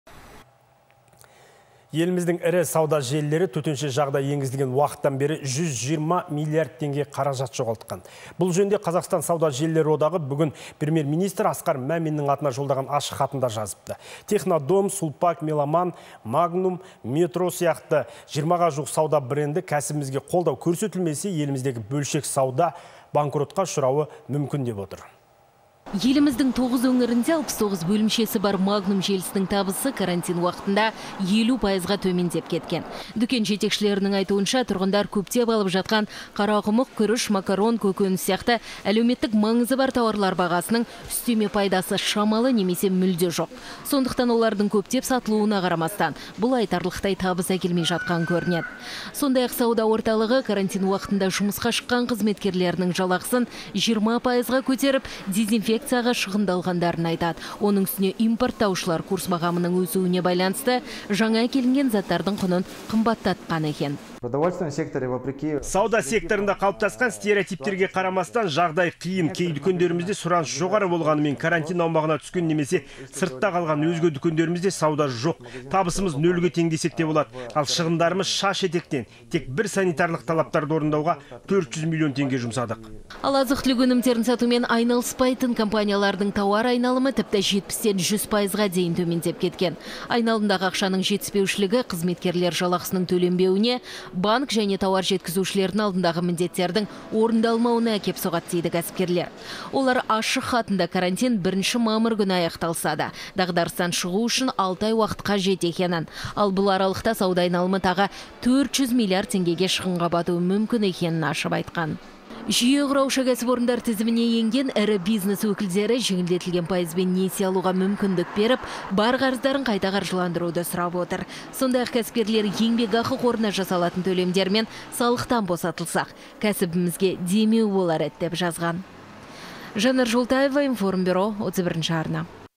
Қазақстандық ірі сауда орындары 120 миллиард теңгеге шығынға батқан
Еліміздің ірі сауда желілері төтенше жағдай енгізілген уақыттан бері 120 миллиард теңге қаражат жоғалтыпты. Бұл жөнінде Қазақстан сауда желілері одағы бүгін Премьер-министр Асқар Мәминнің атына жолдаған ашық хатында жазған. Технодом, Сулпак, Меломан, Магнум, Метро сияқты 20-ға жуық сауда бренді кәсібімізге қолдау көрсетілмесе, еліміздегі бөлшек сауда банкротқа ұшырауы мүмкін -деп отыр.